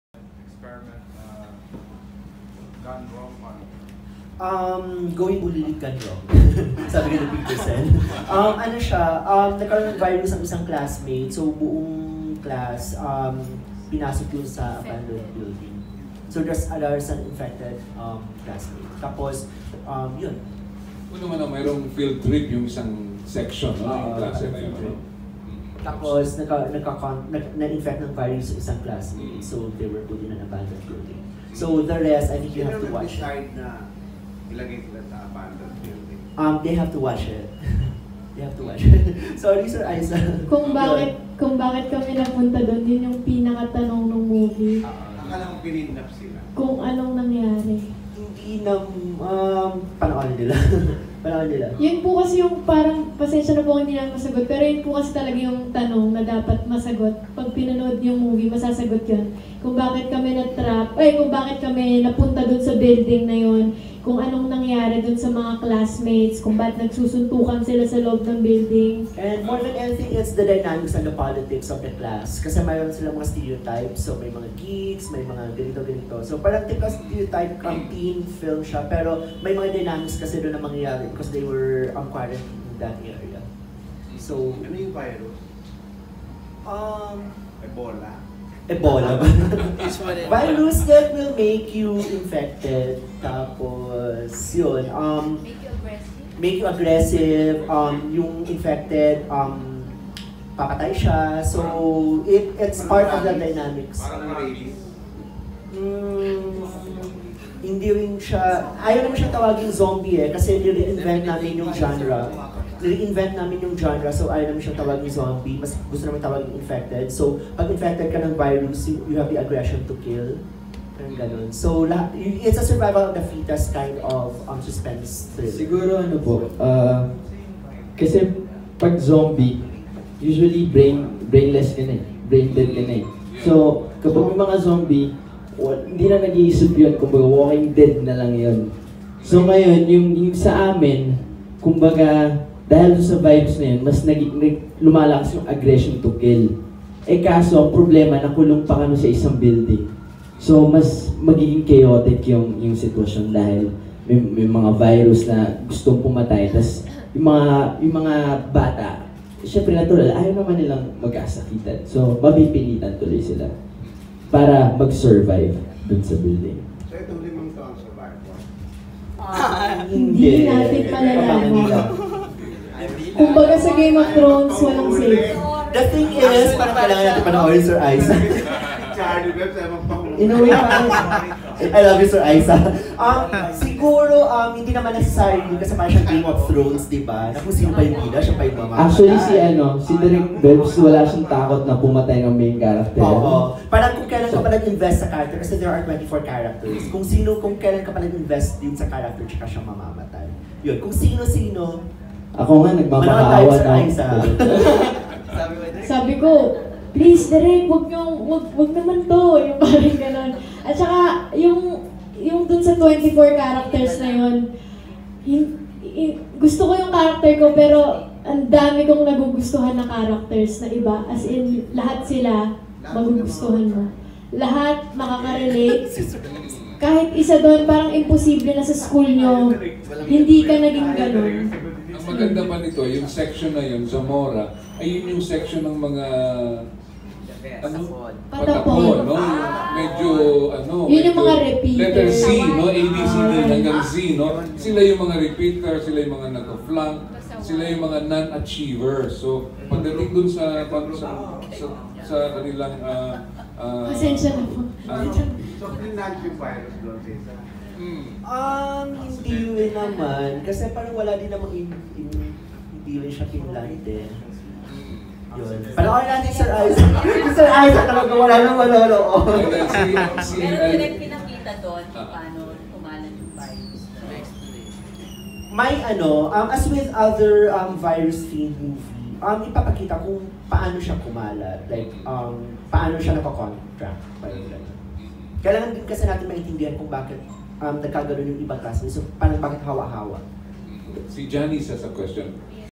Charliebebs Gohetia Talks About His Film Class of 2018
Starring: Nash Aguas, Sharlene San Pedro, CJ Navato, Kristel Fulgar, Kiray Celis
Directed By: Charliebebs Gohetia
Opens In Cinemas Nov 7 2018